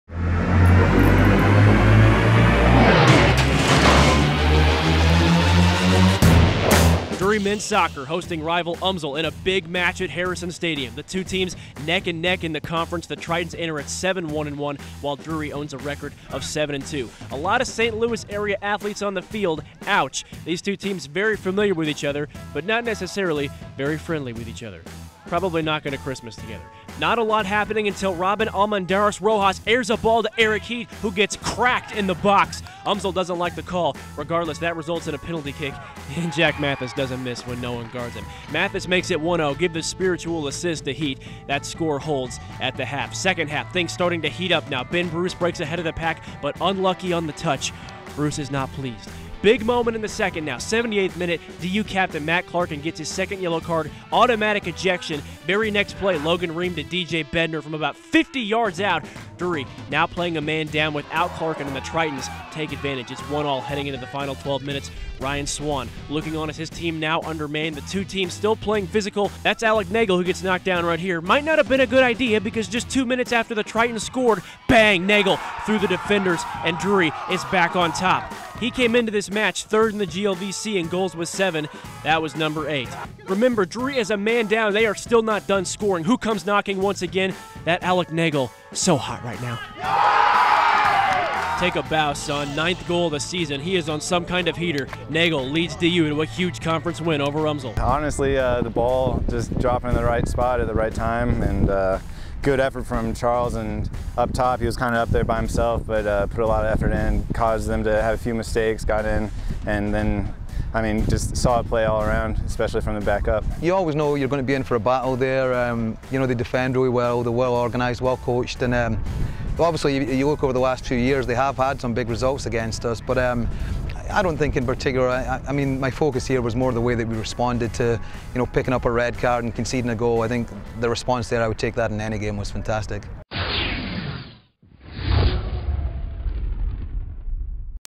Drury Men's Soccer vs. UMSL Highlights [10/5/14]
Drury Men's Soccer gets two goals late from senior Alec Nagel to put away UMSL 3-1 in GLVC play at Harrison Stadium.

Both goals from Alec Nagel came short-handed after Drury's Matt Clarkin was ejected due to a red card in the 78th minute. Nagel now has 9 goals on the season to lead Drury.